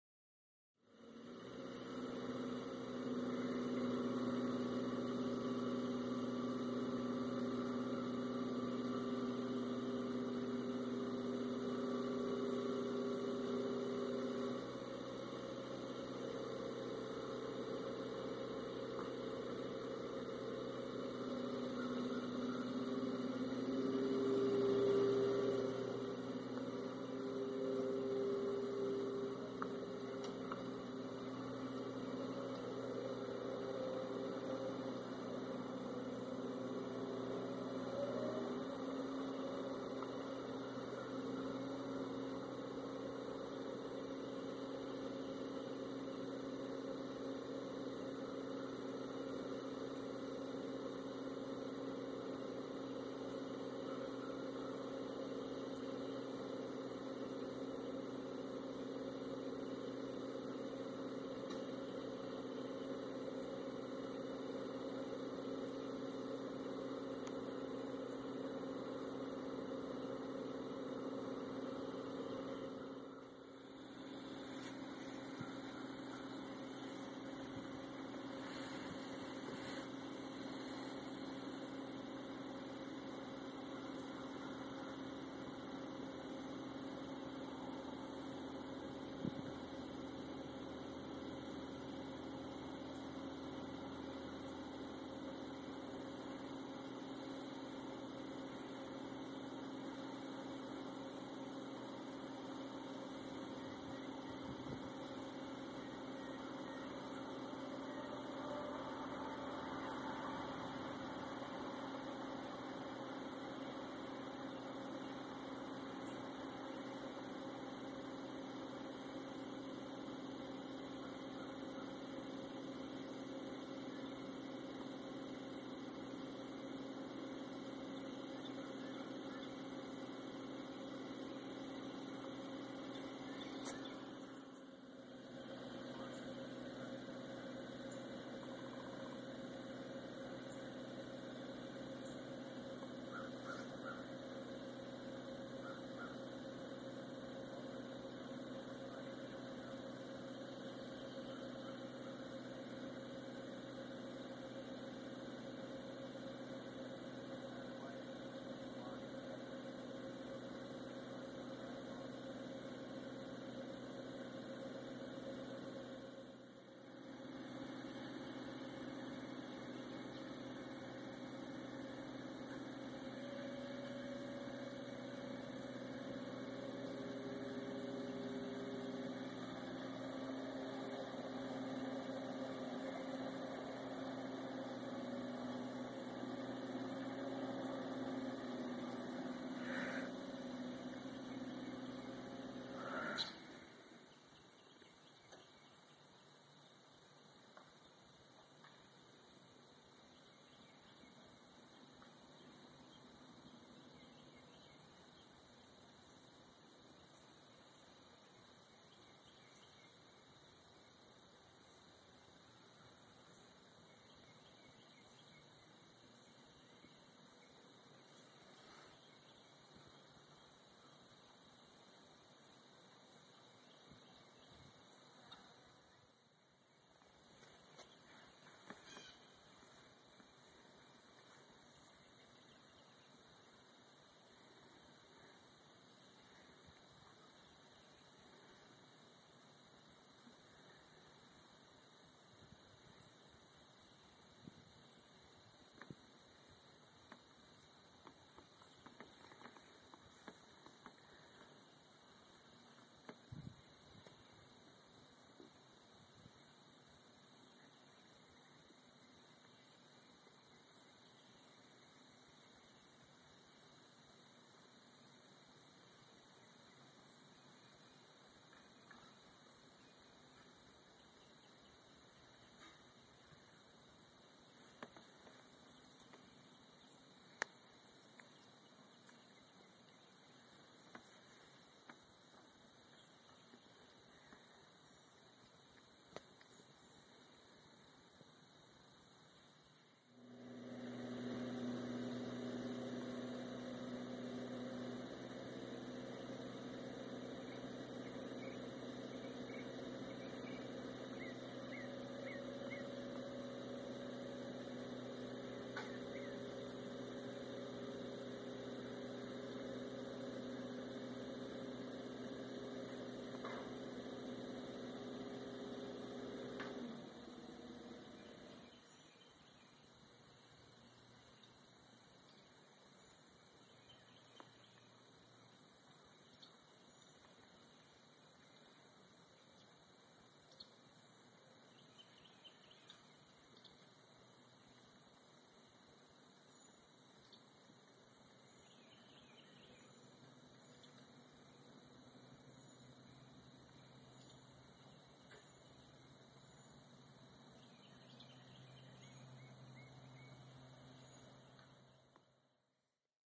Netting bees
Netting a load of bees headed for North Dakota at 6am